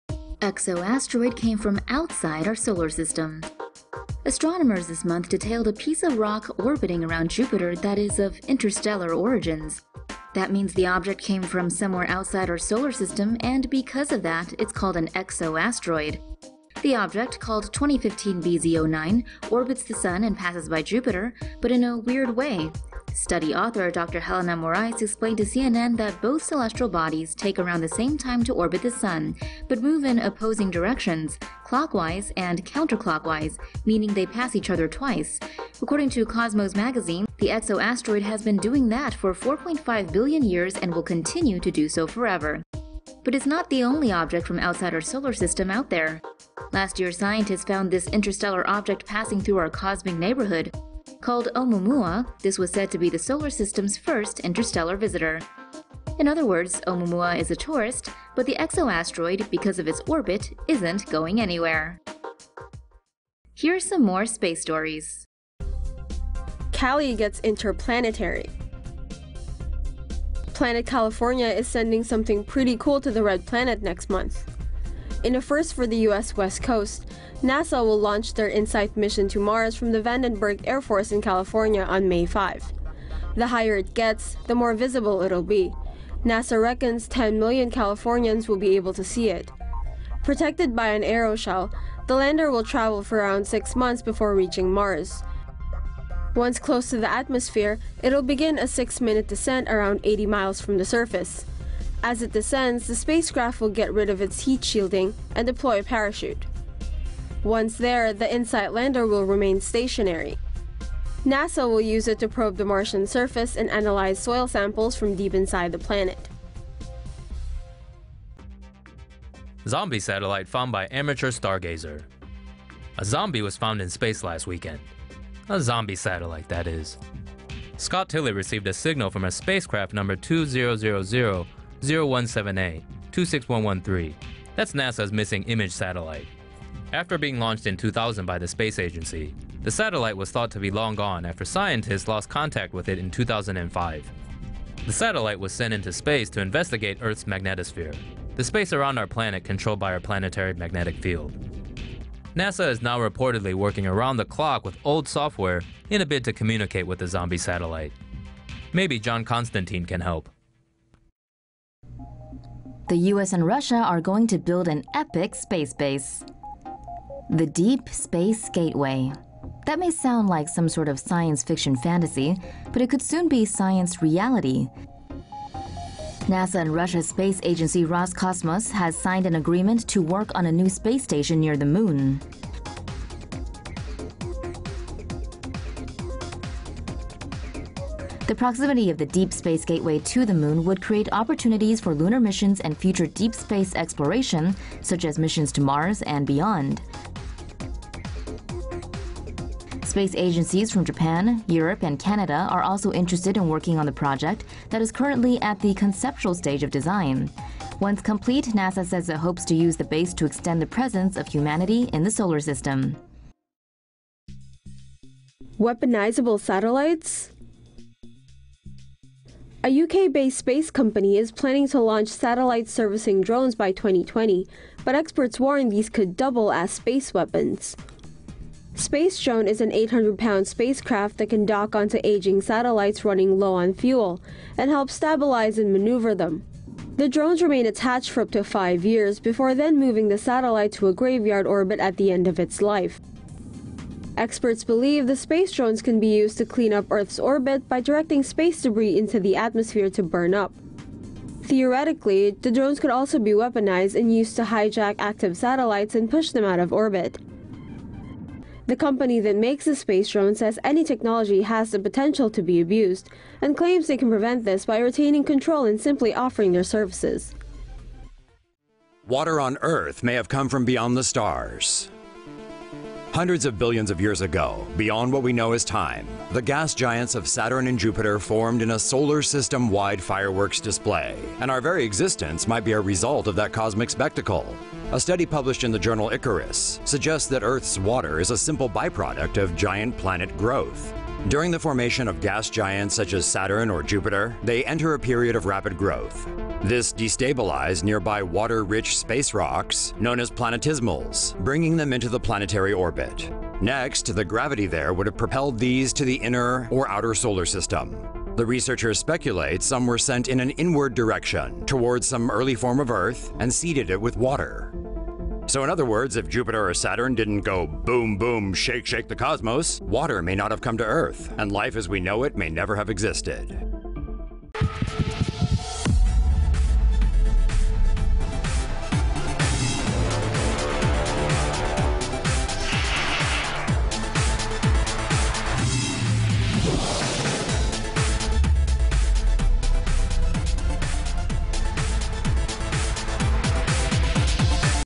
Interstellar exo-asteroid is form beyond our solar system - TomoNews
THE COSMIC NEIGHBORHOOD — Astronomers this month detailed a piece of rock orbiting around Jupiter that is of interstellar origins.

That means the object came from somewhere outside our solar system and because of that, it's called an 'exo-asteroid.'

The object, called 2015 BZ09, orbits the sun and passes by Jupiter, but in a weird way. Study author Dr. Helena Morais explained to CNN that both celestial bodies take around the same time to orbit the sun, but move in opposing directions: clockwise, and counterclockwise, meaning they pass each other twice.

According to Cosmos Magazine, the exo-asteroid has been doing that for 4.5 billion years and will continue to do so forever.

Last year scientists found this interstellar object passing through our cosmic neighborhood. Called Oumumua, this was said to be the solar system's first interstellar visitor.

In other words, Oumumua is tourist, but the exo-asteroid, because of its orbit, isn't going anywhere.

Details of the exo-asteroid was published in the journal, Monthly Notices of the Royal Astronomical Society: Letters.